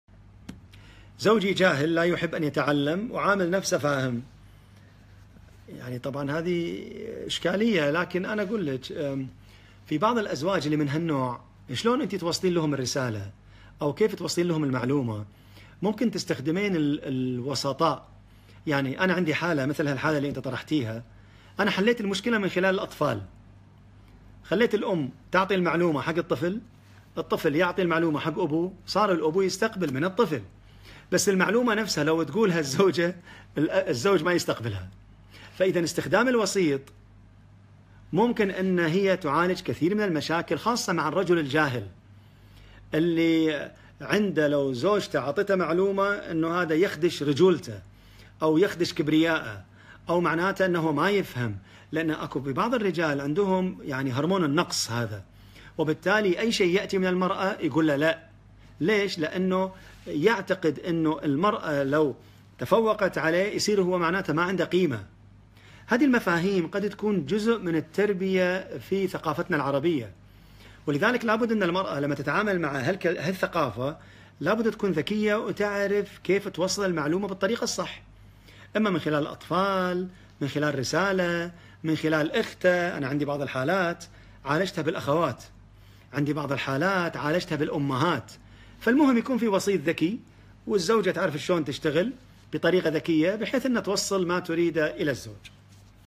زوجي جاهل ويرفض أن نخبره أي معلومة
زوجي جاهل ويرفض أن نعلمه ونطوره ونخبره أي معلومة